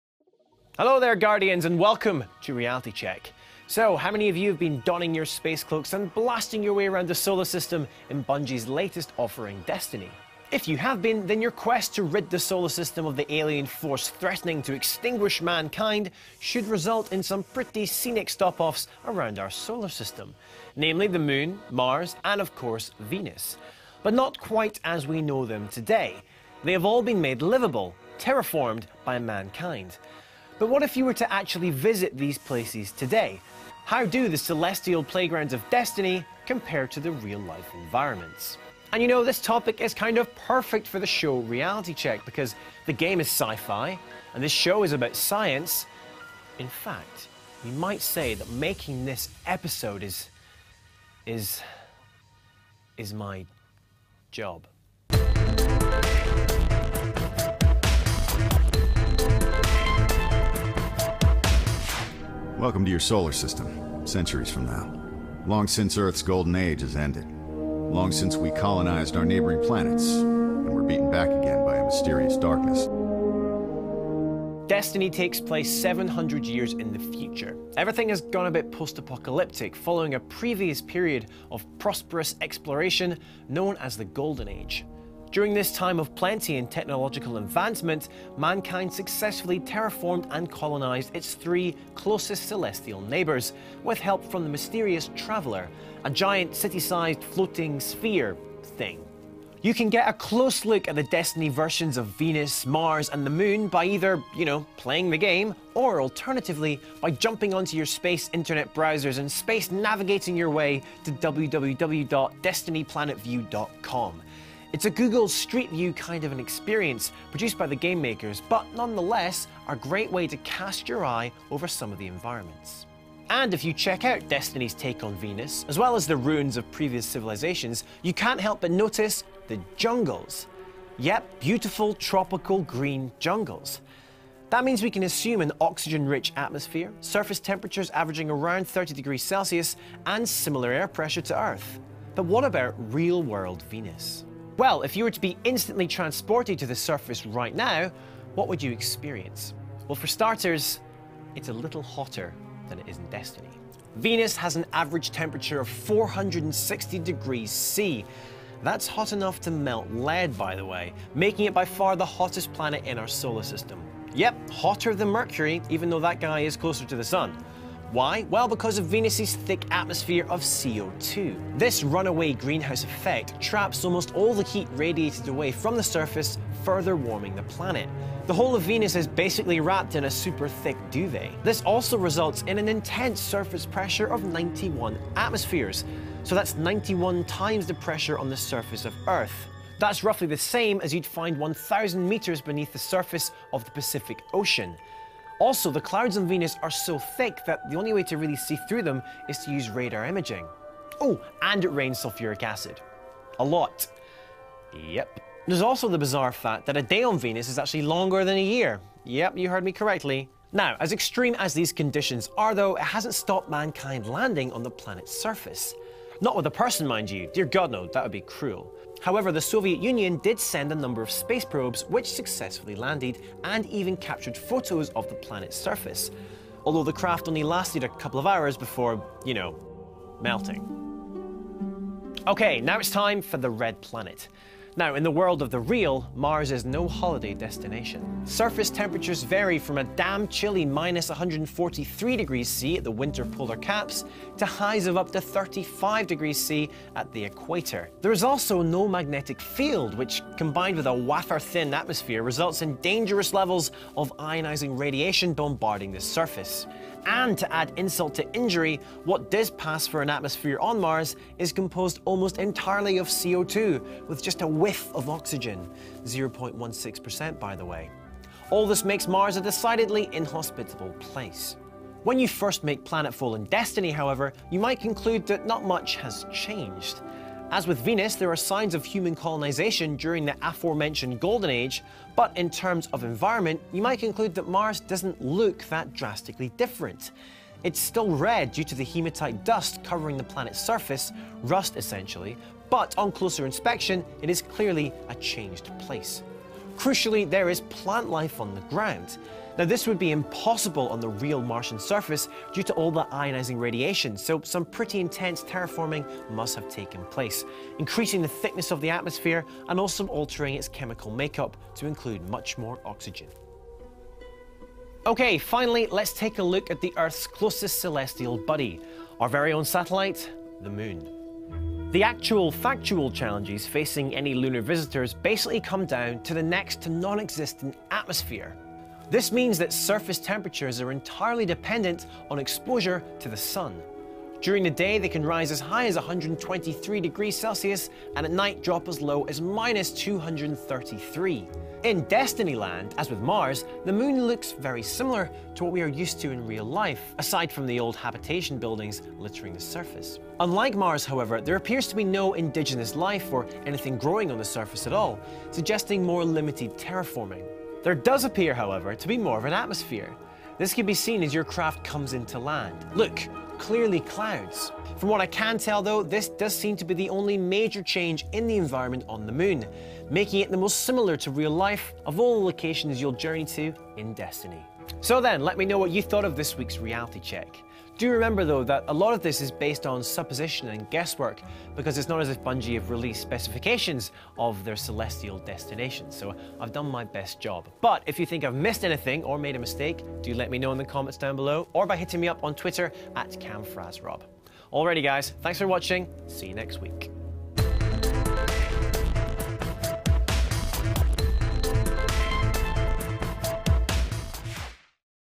Can You Live on Destiny's Planets? - Reality Check
Cam investigates how close Destiny comes to recreating Mars, Venus, and the Moon. Could we ever colonize these planets, or are they totally inhospitable?